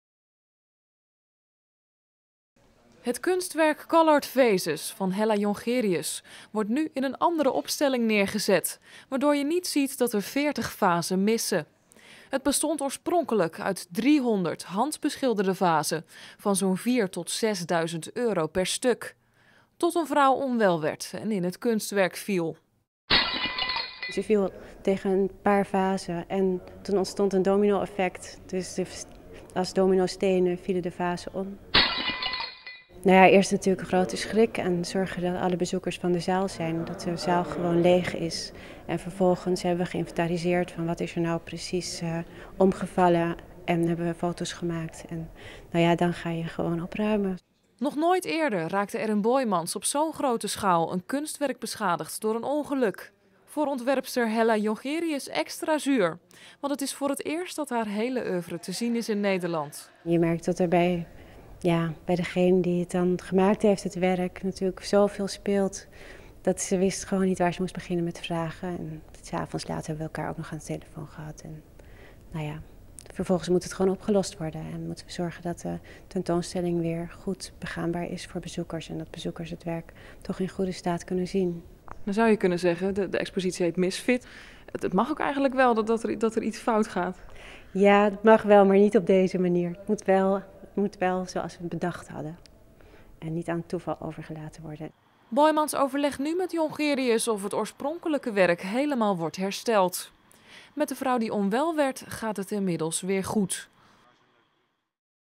Kunstwerk Boijmans beschadigd na val bezoekster
In Museum Boijmans van Beuningen in Rotterdam is dit weekend een kunstwerk zwaar beschadigd nadat een bezoekster er overheen viel. De vrouw was onwel geworden. De schade bedraagt meer dan 100.000 euro. Het kunstwerk Coloured Vases van Hella Jongerius bestond uit driehonderd handbeschilderde vazen van Koninklijke Tichelaar Makkum.